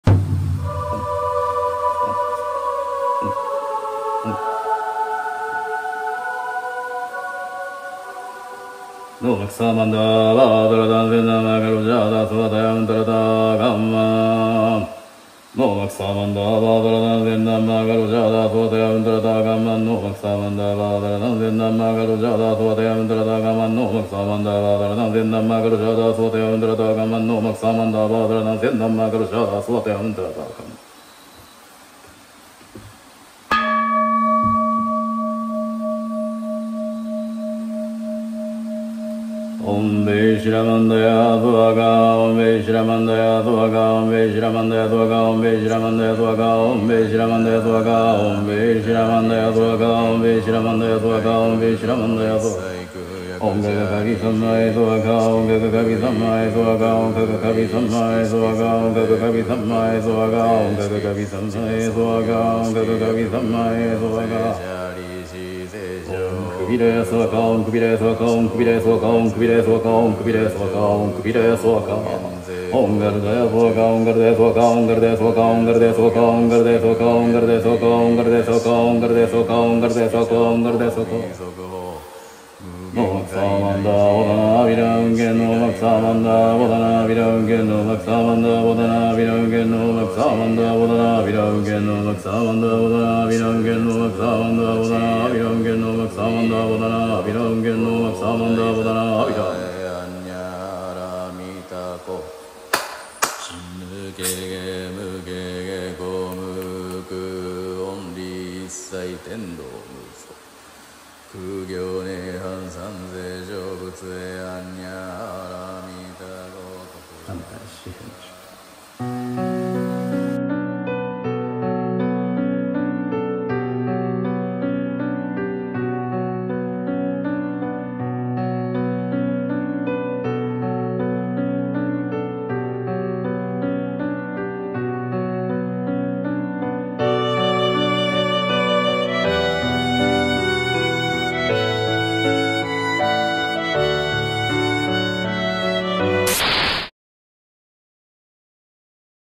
聴き流すだけで運気があがります。spiritual healing movie
皆様へ日頃の感謝を込めて
開運波動を流します

いつも頑張っているあなたへ
私はいつもお祈り致しておりますよ

神様のお導きがございますように
トホカミエミタメ

spiritual  artist SHO